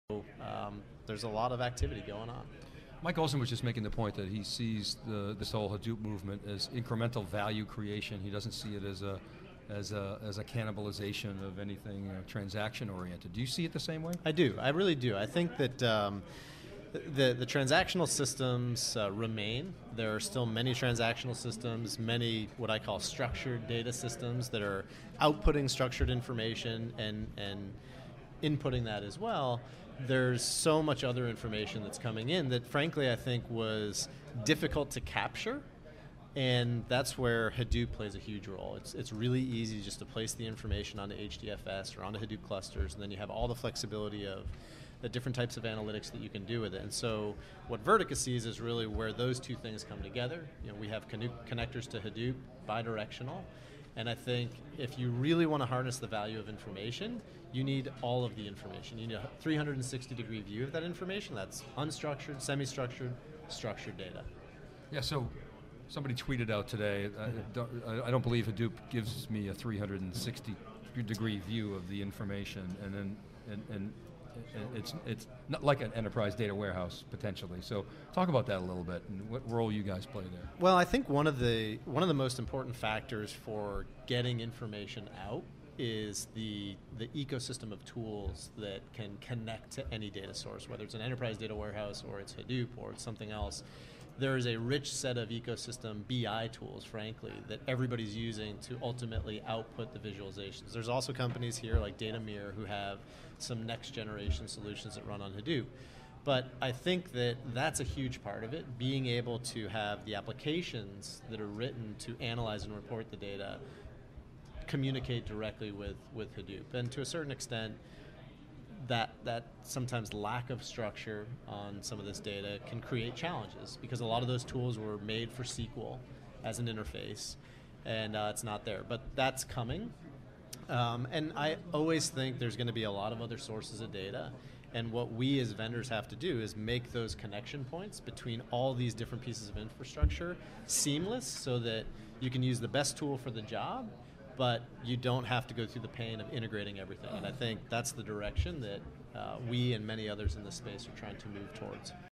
Vendors' Job to Create Seamless Data Connections, Says HP Vertica's Mahony
Hadoop and high-performance databases are complimentary to transactional ... HP Vertica's Colin Mahony live inside theCUBE at Hadoop World 2011 in New York City.